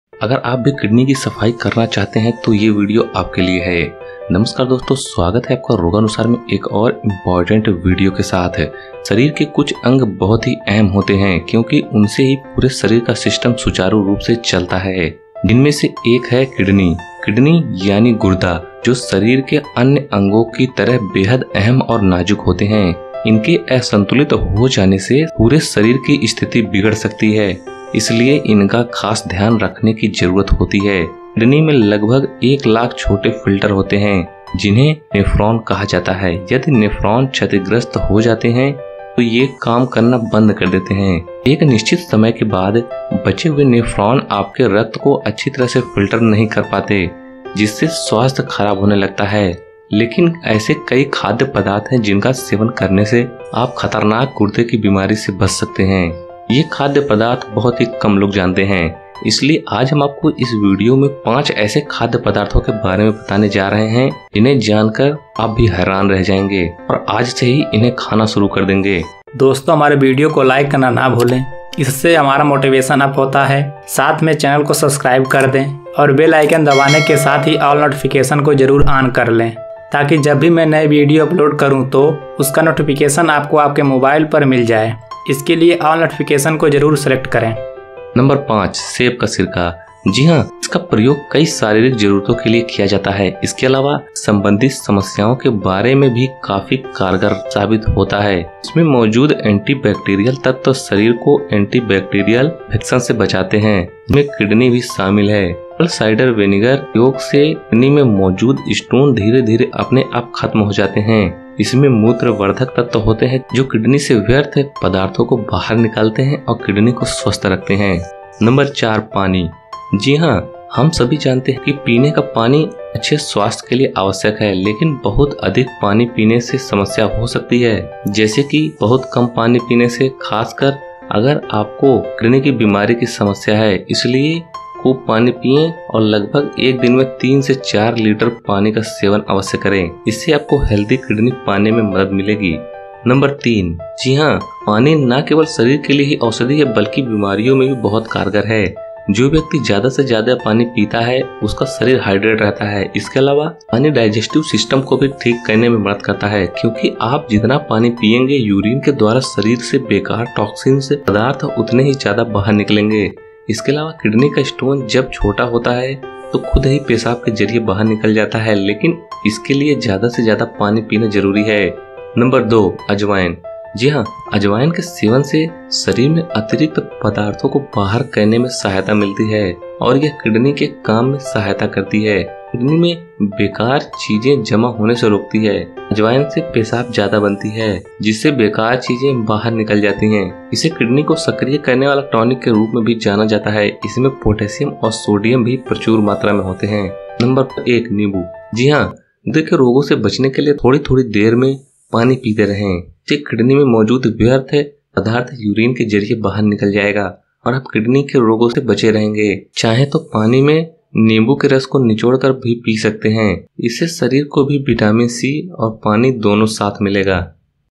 7 दिन लगातार इसे पी लो किडनी हो जायेगी बिल्कुल साफ। बिलकुल हो जाएगी छोटे बच्चे जैसी। LIVER DETOXIFY
7 दिन लगातार इसे पी लो किडनी हो जायेगी बिल्कुल साफ। बिलकुल हो जाएगी छोटे बच्चे जैसी। LIVER DETOXIFY